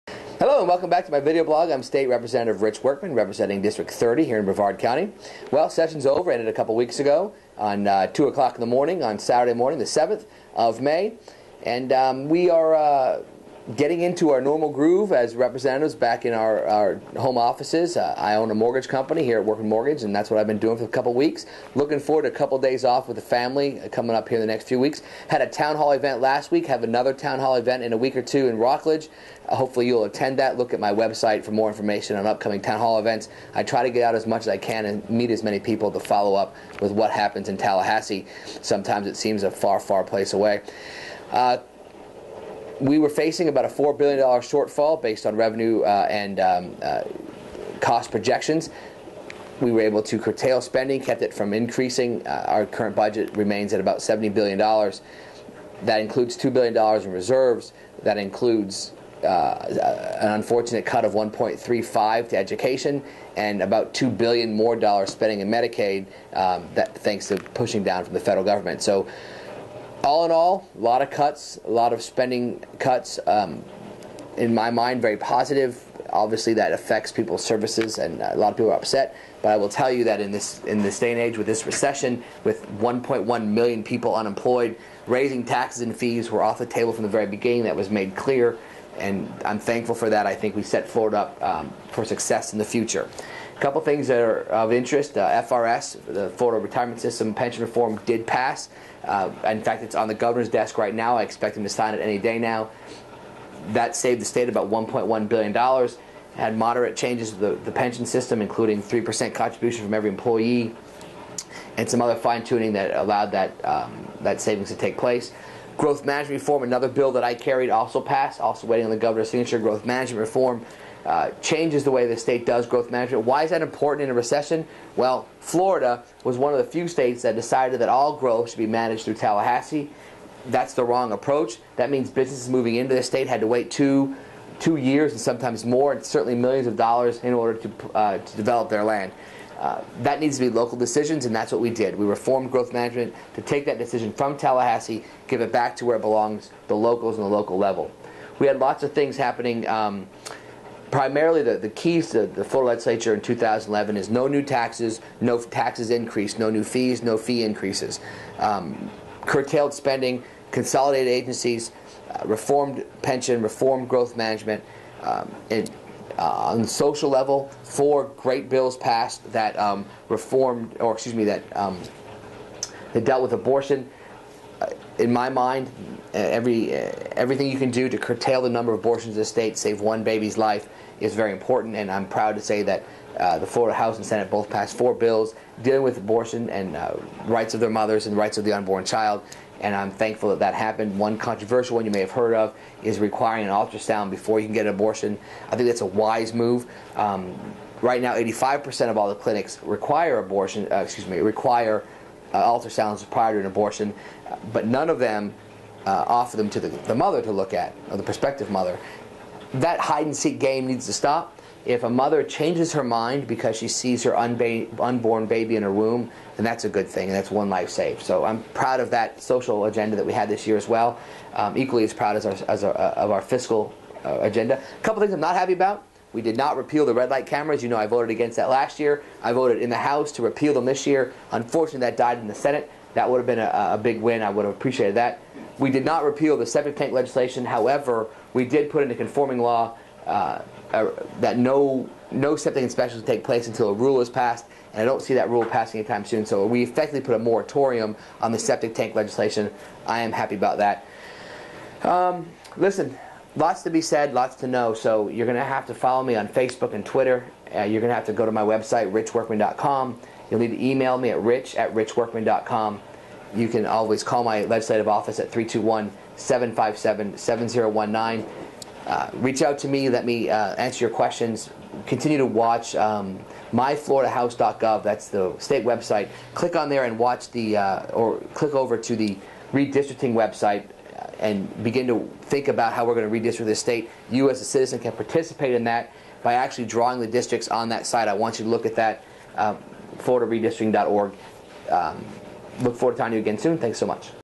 Rep Ritch Workman Video Blog 9
Florida House member Ritch Workman discusses the end of the 2011
legislative session.